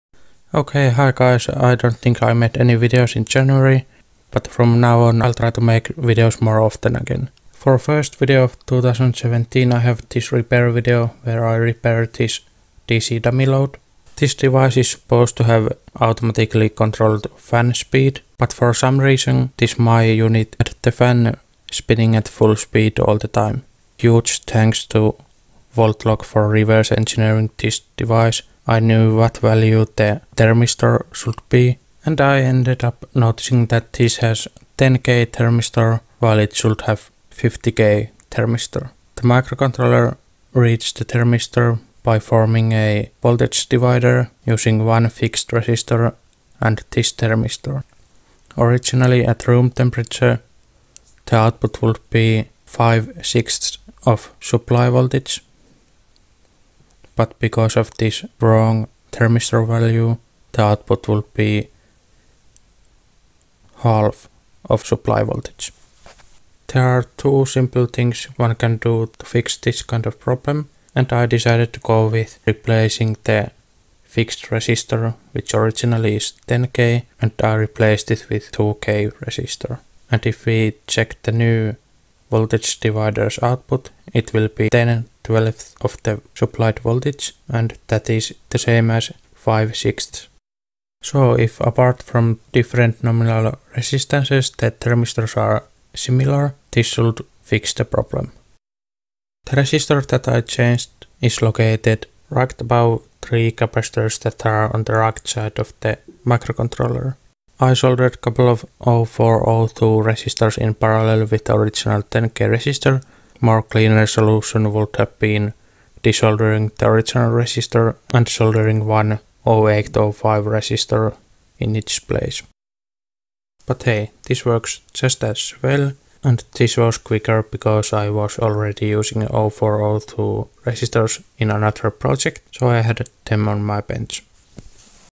DC constant Current Load's "Always ON" Fan - How to Fix it!
My adjustable DC constant current load was supposed to have "Intelligent fan control", but the fan was always spinning at full speed. In this video I show what I did to fix that problem.

Thanks to VoltLog's video about reverse engineering this dummy load I was able find the cause of this problem and fix it in couple of minutes.

VoltLog pointed out to another viewer who had this same problem, that the faulty unit might be clone because of differences in silk screen. My unit doesn't have the "ZHIYU" text either, so this might be a clone of the original design.
I searched "constant current load 9.99a" on eBay and selected the cheapest one... If this faulty thermistor is only problem, I'm happy with my purchase.